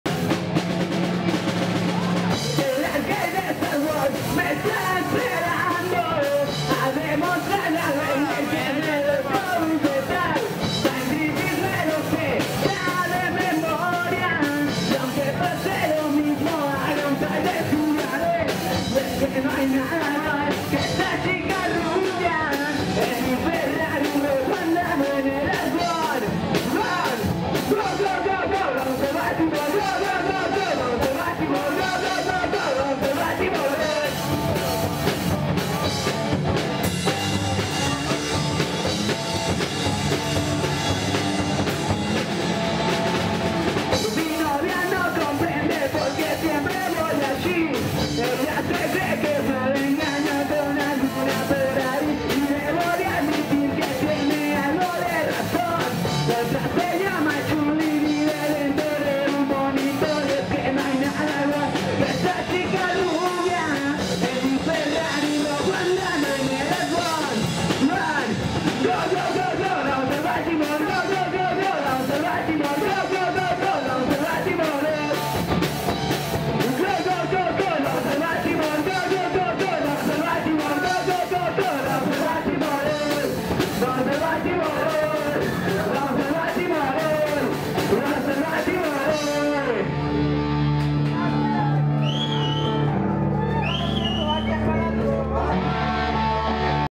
BOLSILLOS VACÍOS EN EL CAFÉ THE ART
Presentación de la banda de punk rock Bolsillos Vacíos en el Café the Arte en Valle de Chalco el sábado 27 de agosto en el evento Es solo punk rock